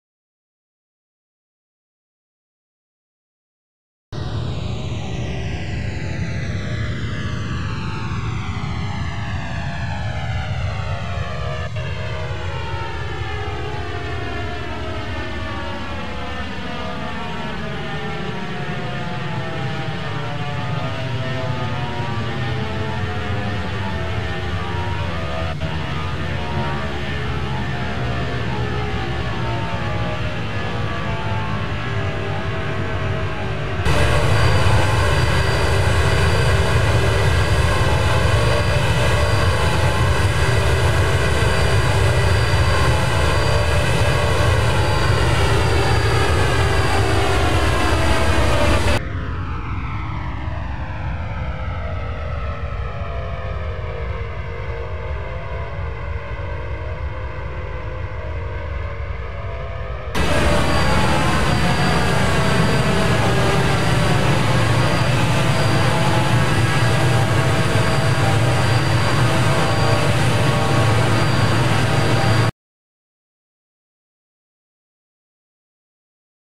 United 757 Departing ORD
United Airlines 757-200 Departing Chicago's O'hare Airport